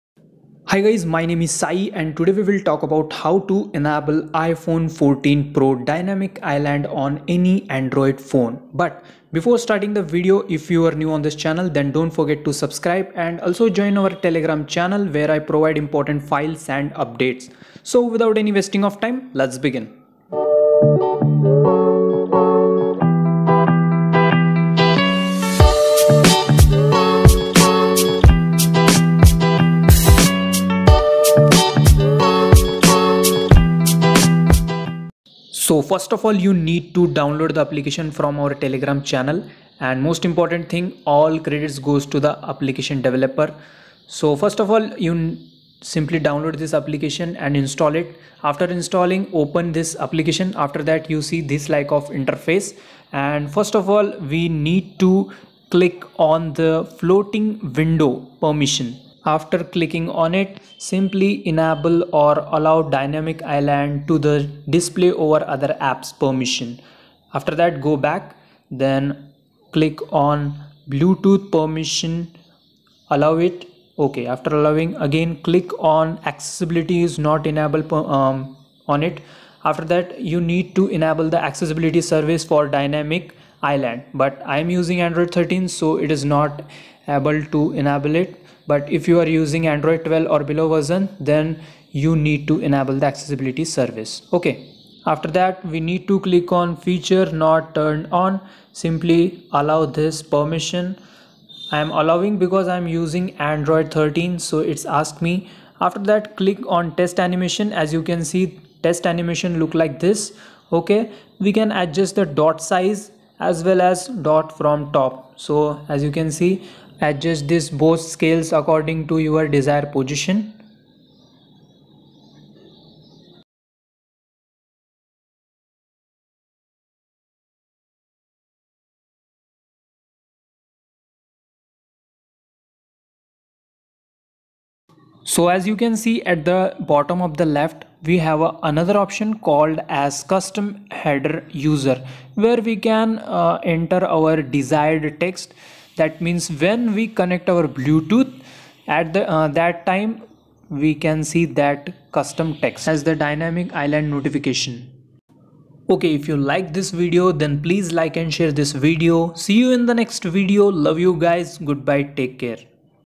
How To Enable Iphone 14 Pro Dynamic Island On Android Phone.How To Install Dynamic Island On Android
(★) TG Channel (New User Step By Step Guide + All IMP Files) :-

Sai Ponnamanda